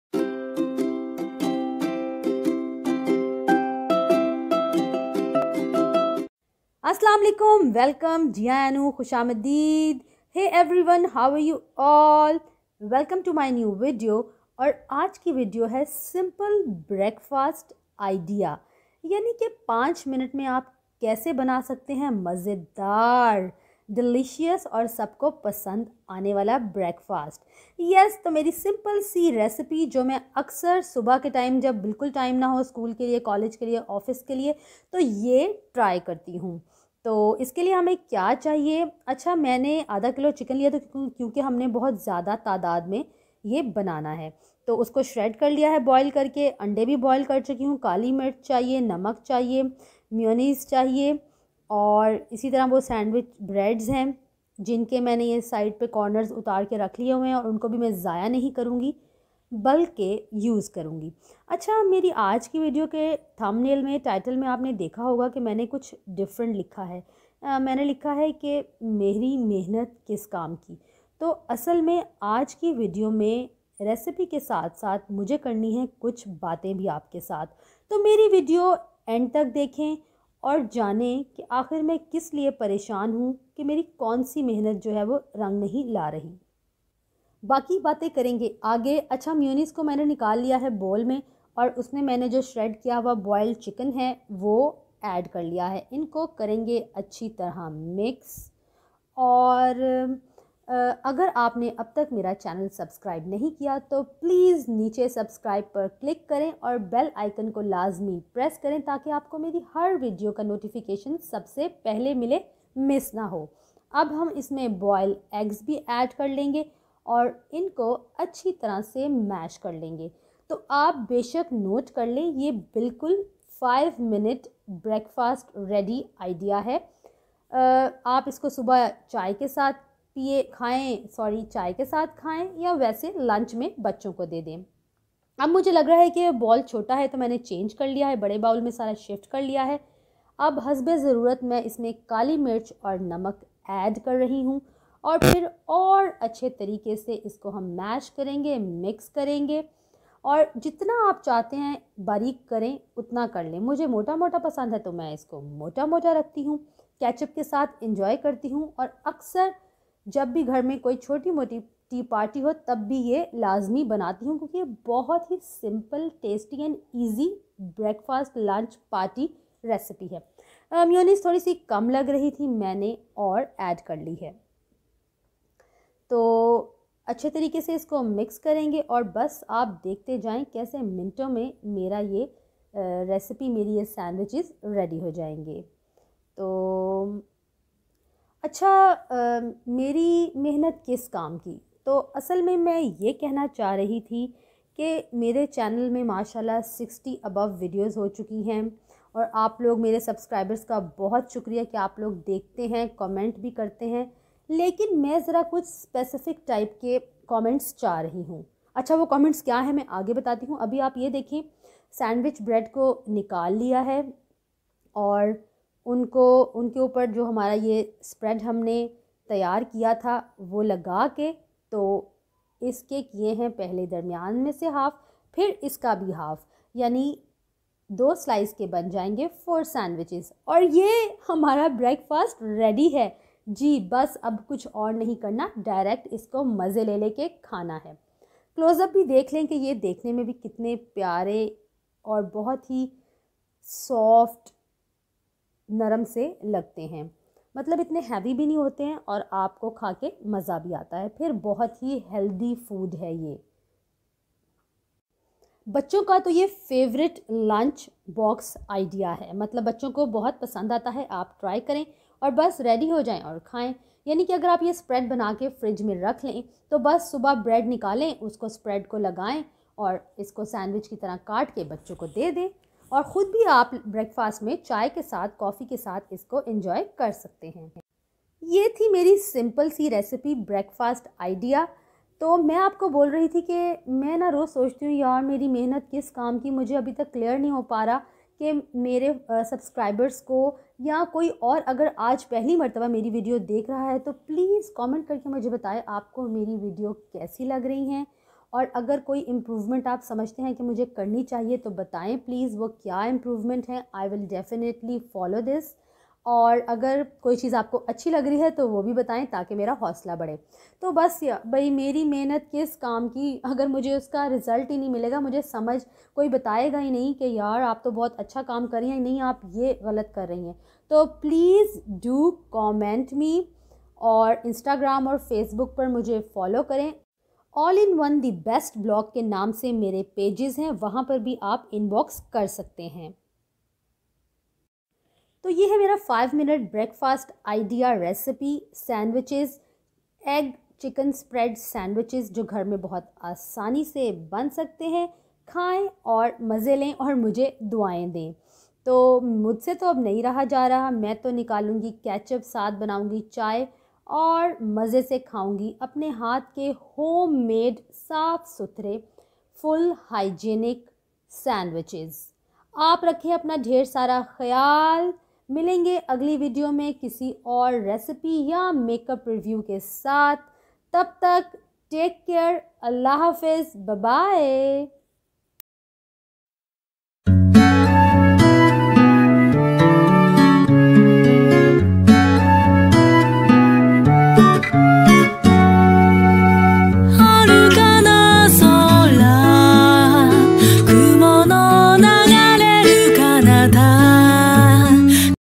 My 5 mints Breakfast Recipe | Lunch Box Ideas for Kids | Egg Mayonnaise Sandwich | Heart Talkies
Hello everyone!
Here is my easy and instant 5 Minute Breakfast Sandwich recipe that will save your mornings. I have shared some Dil ki batain also.. Watch and give your feedback

 Ingredients:
1. shredded chicken
2. Mayonnaise
3. Salt
4. Black Pepper
5. Bread
6. Boiled Eggs